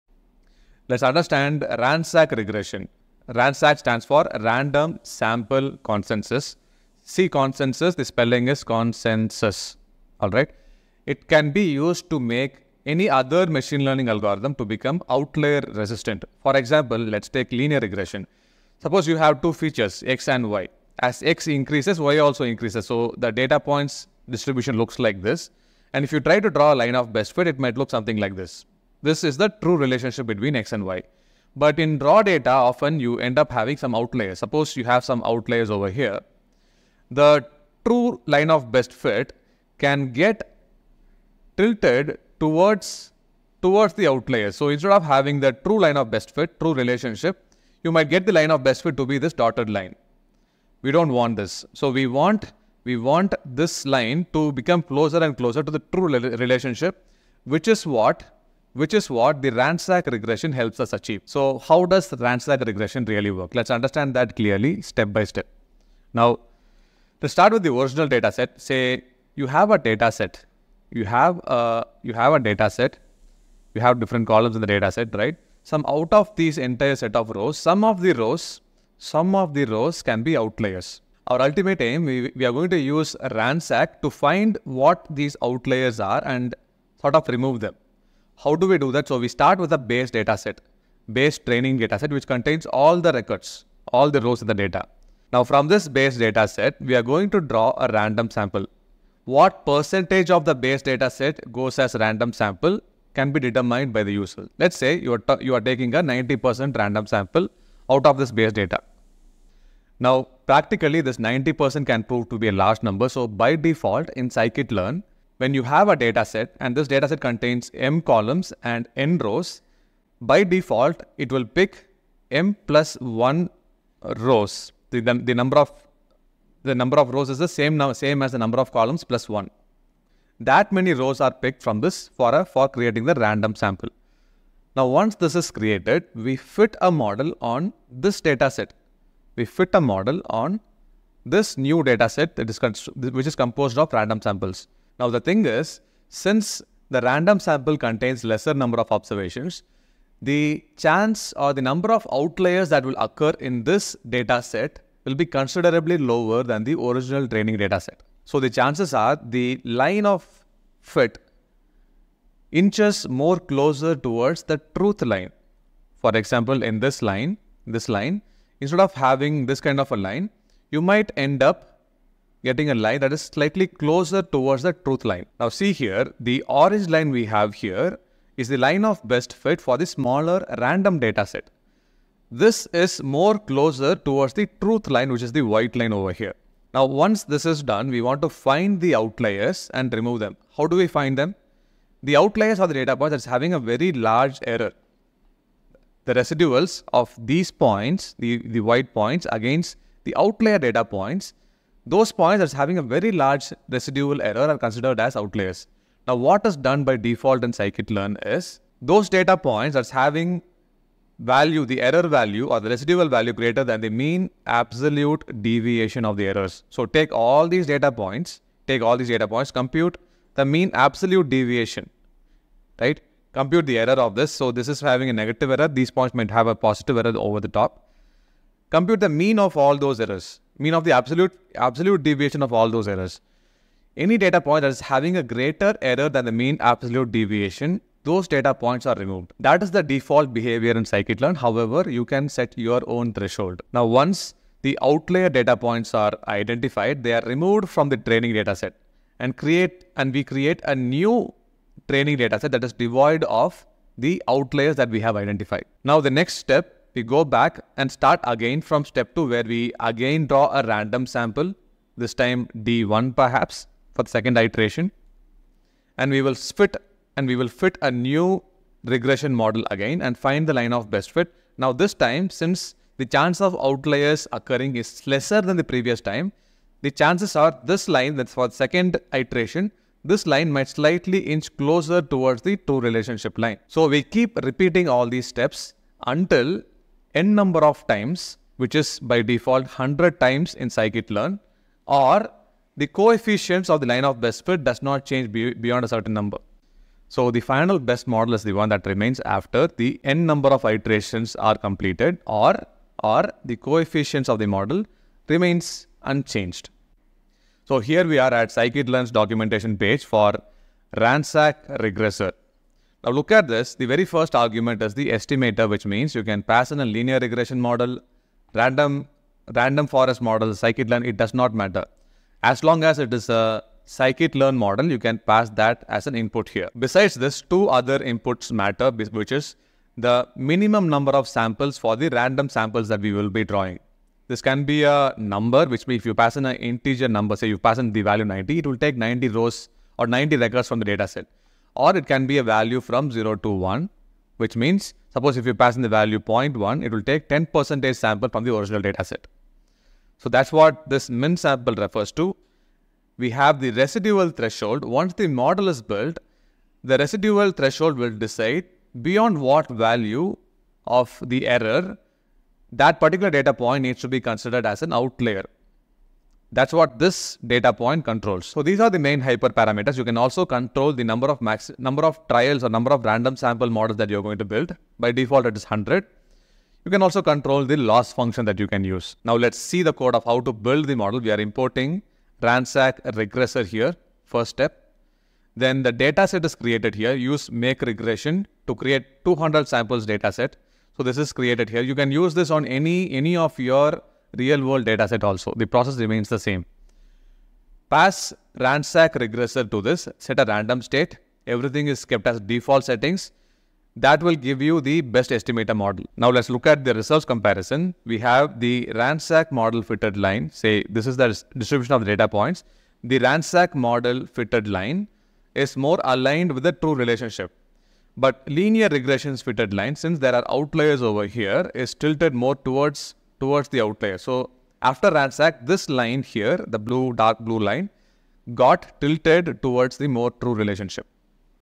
RANSAC - Outlier Resistant Regression, Algorithm Clearly Explained
In this video, we'll see RANSAC Regression, short for Random Sample Consensus. It can be used to make other ML models to become outlier resistant. For example, the line of best fit for linear regression is highly sensitive to outliers.

By using RANSAC algorithm, we can ensure, the trained Linear Reg model becomes resistant outliers.

Let's understand how RANSAC algorithm works very clearly in this one.

Chapters

00:00 Introduction
01:06 How RANSAC Works
02:55 Removing Outliers
03:26 Threshold Rules
04:44 Stopping Criteria
05:07 RANSAC Regressor Parameters
06:46 Results Comparison

Join ML+ membership for exclusive Data science content